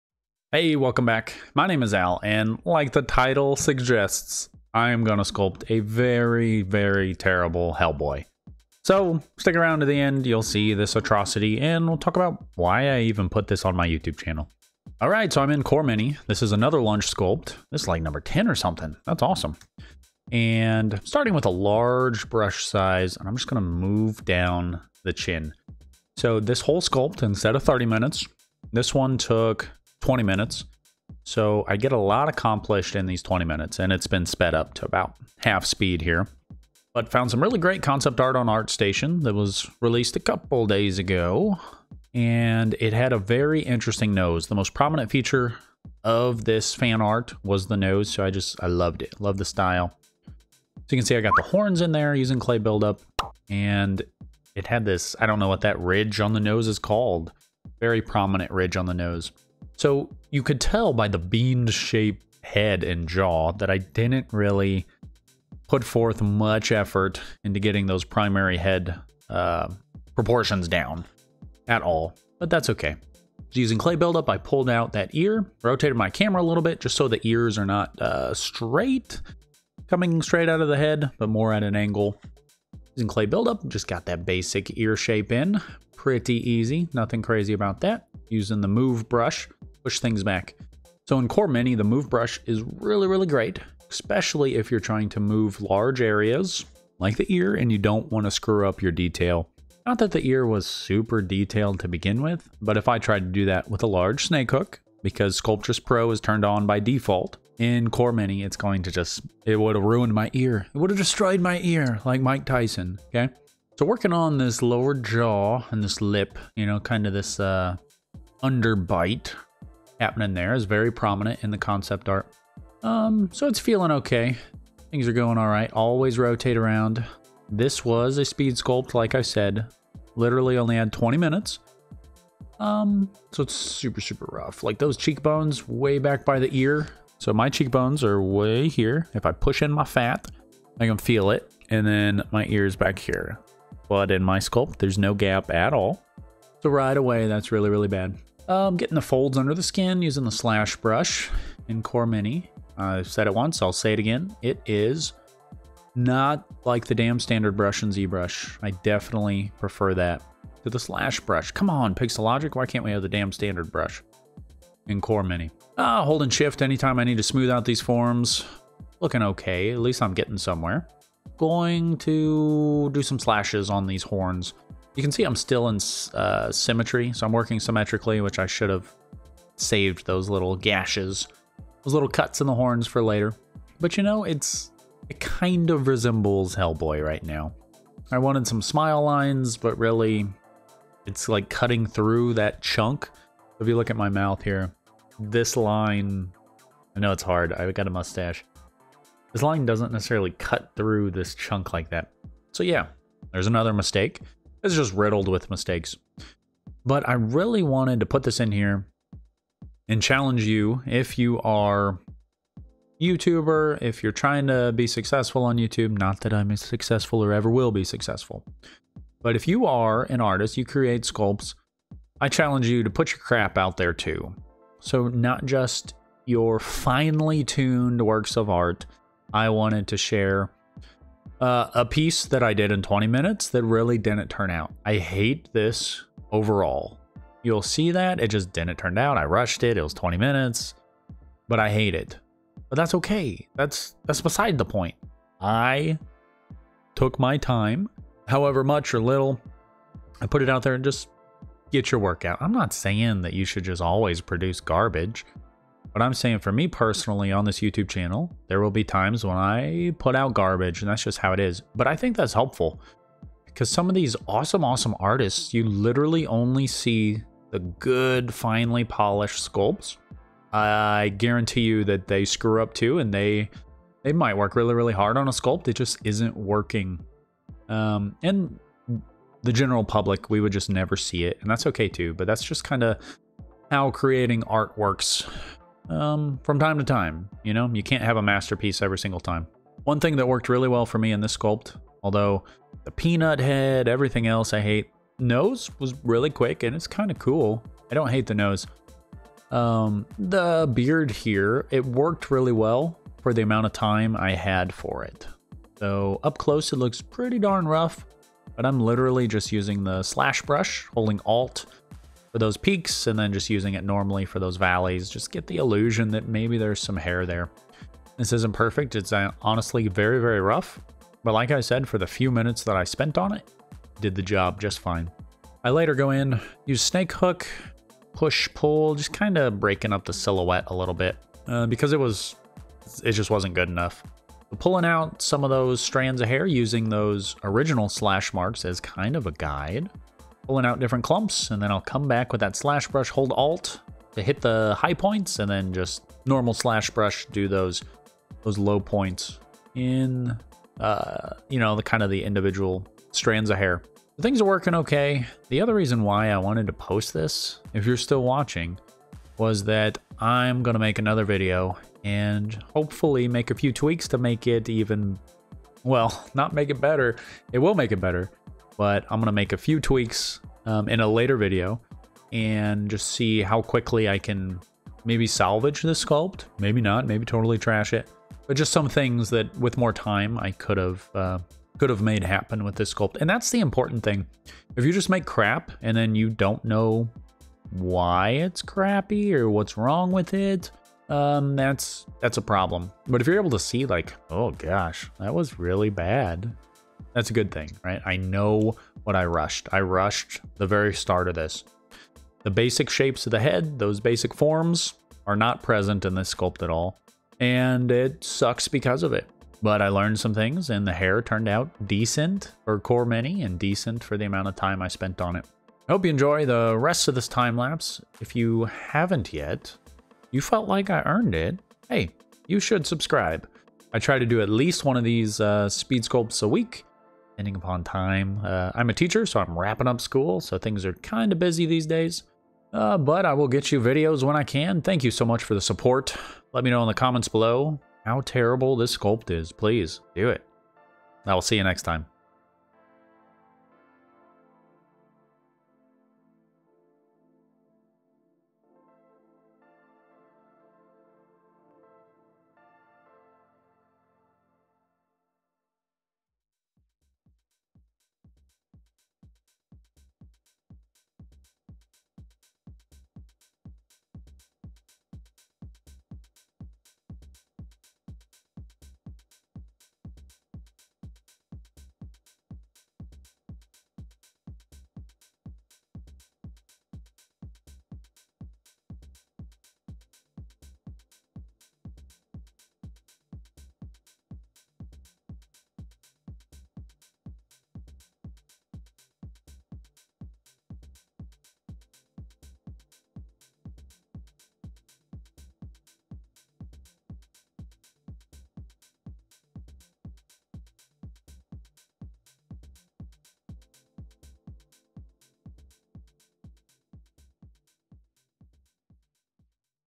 Why I Post My Terrible 3d Art on YouTube... And Why You Should Too...
We've all been there... You sit down to sculpt or create art and it just isn't working. It can be a struggle or the end result might be terrible.
In this video I discuss why it my be a GOOD thing to post your failed art attempts.

The bad days make the good days better... Happy Sculpting!